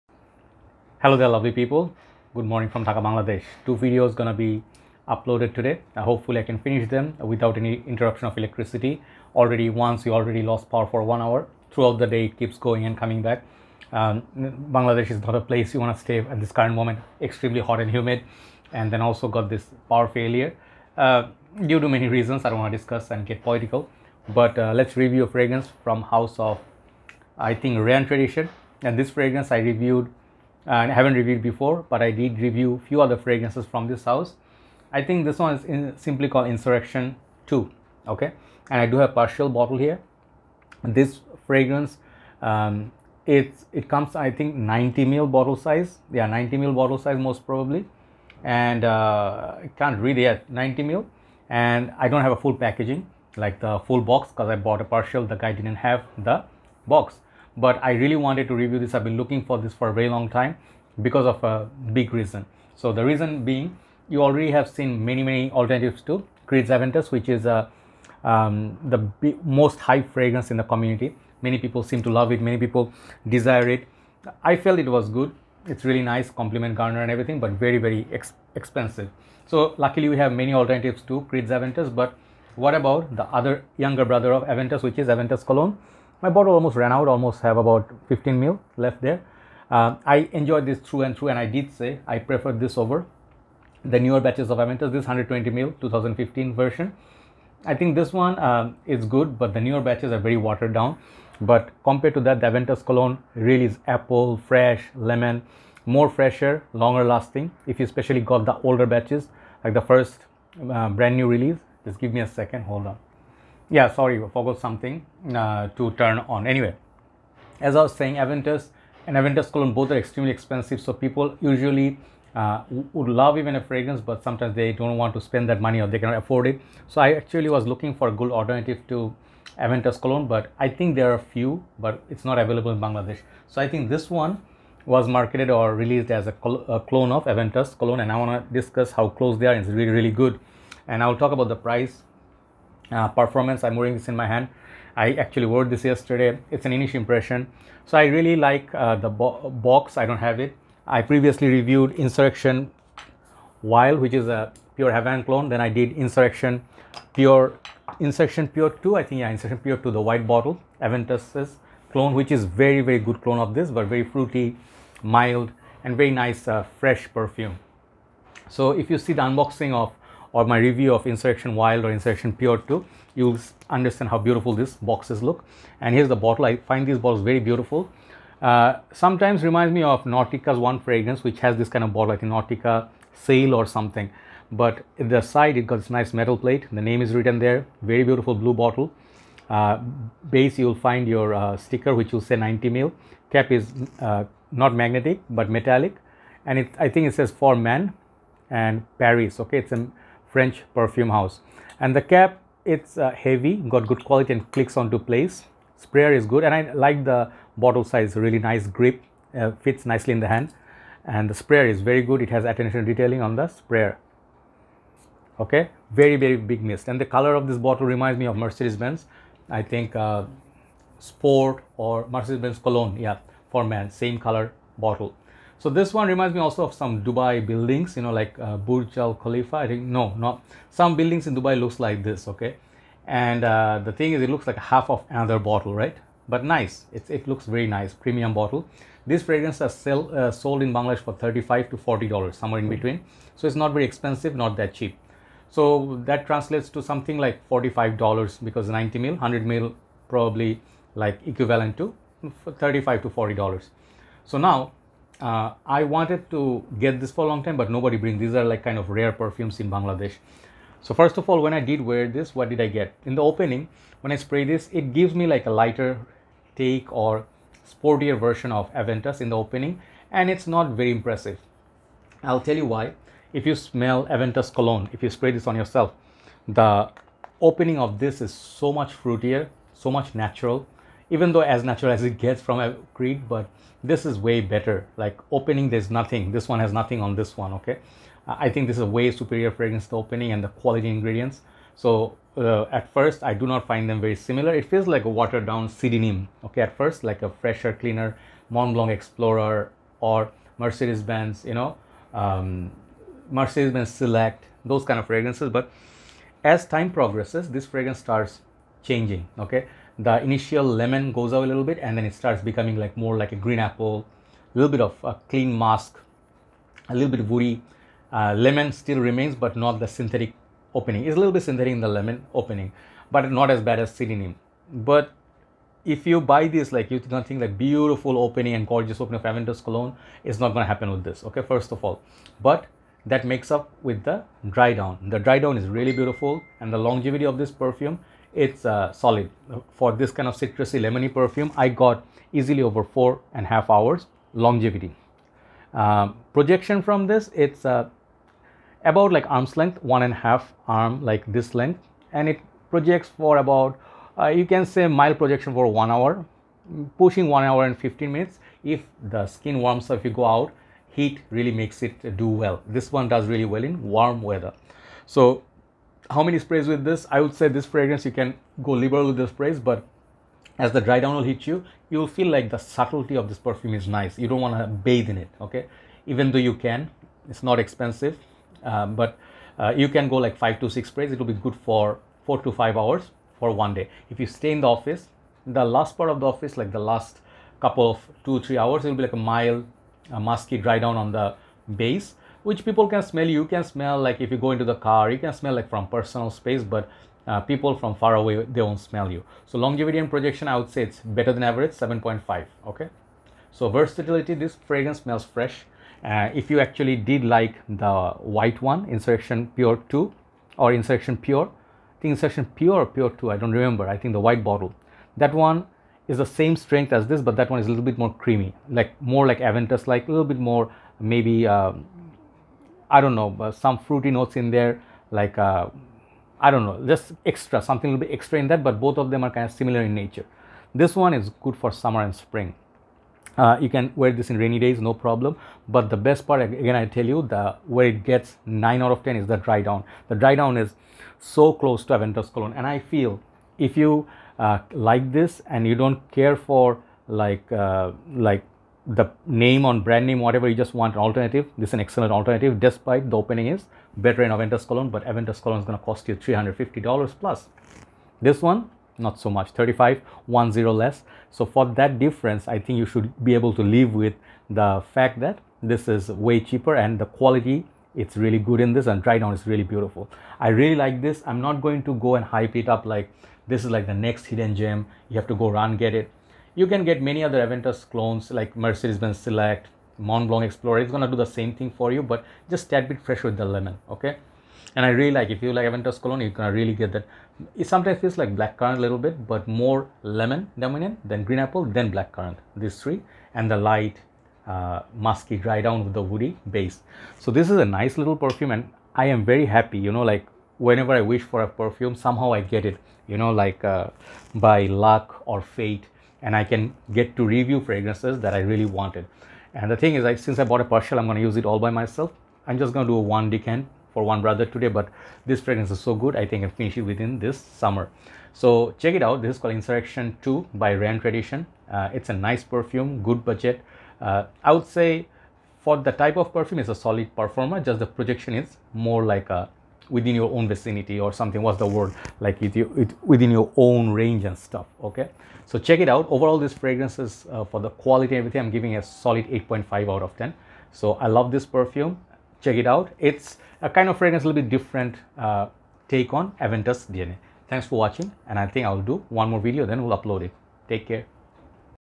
Reyane Tradition Insurrection II Sport Review (2021)
This is an Creed Aventus Cologne clone, my sincere apologies as I said this one is Insurrection II in my video, I bought a partial which came with no box. anyway the review is ok lol. check it out if you liked Aventus Cologne but it;+'s too pricey for your liking.